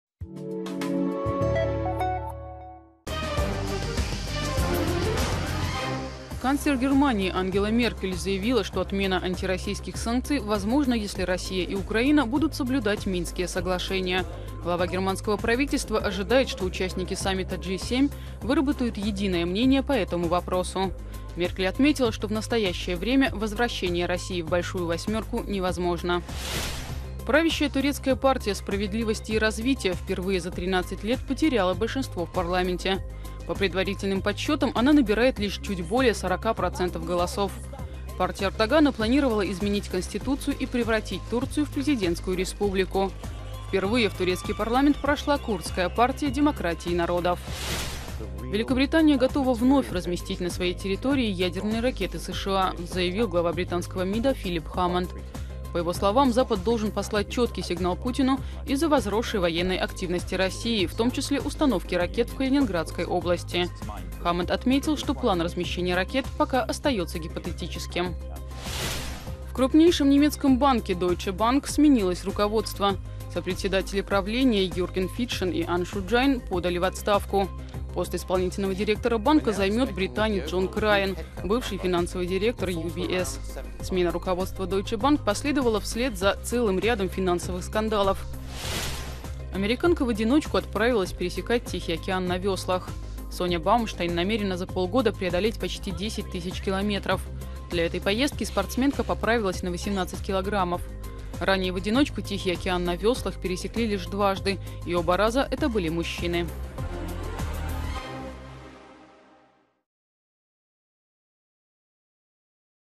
Геофактор коротко: 08.06.2015 15:00 (мск)
Меркель назвала условия отмены санкций против России. Партия Эрдогана потеряла большинство в парламенте. Великобритания готова разместить ядерные ракеты США.